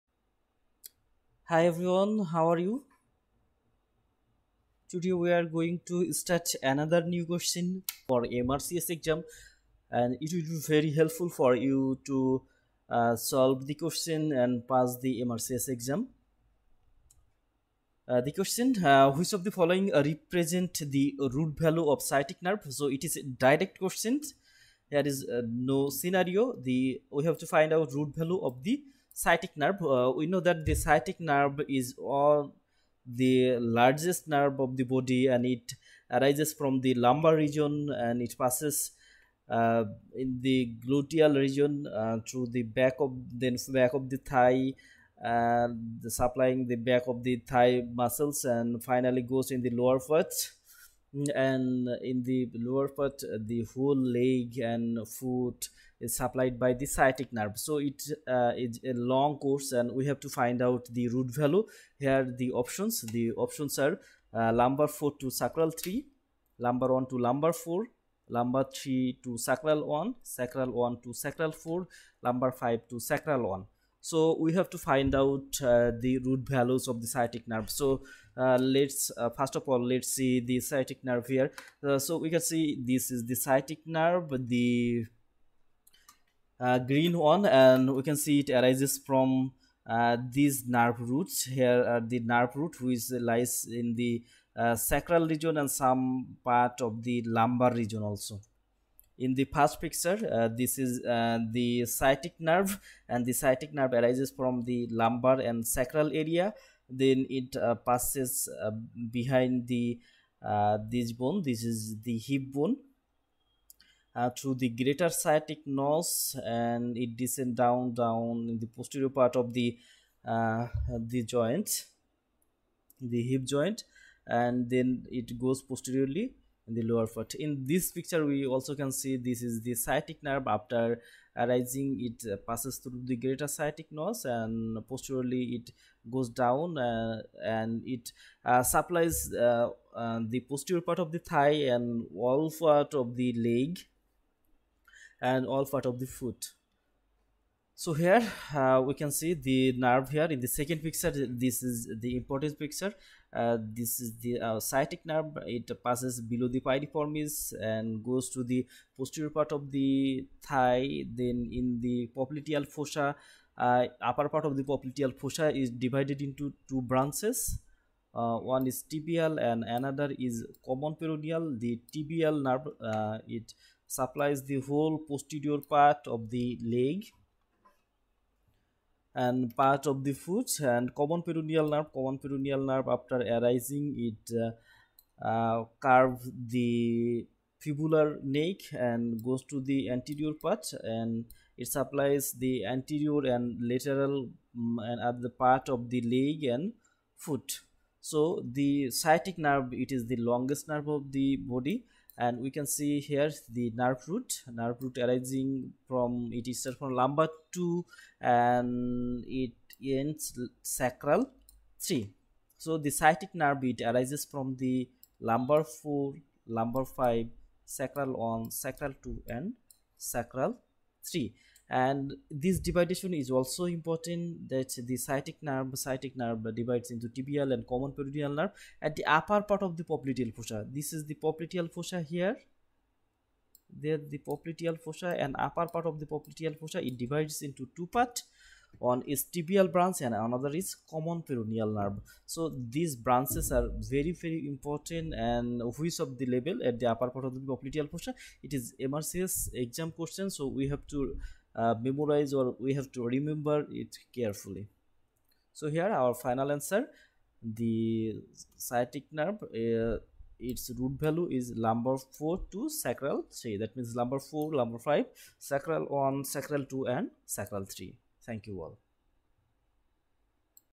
Sciatic nerve at gluteal region MRCS question solve
it includes
1. eMRCS videos
2. Fawzia sheet solved
3. Recalls and mores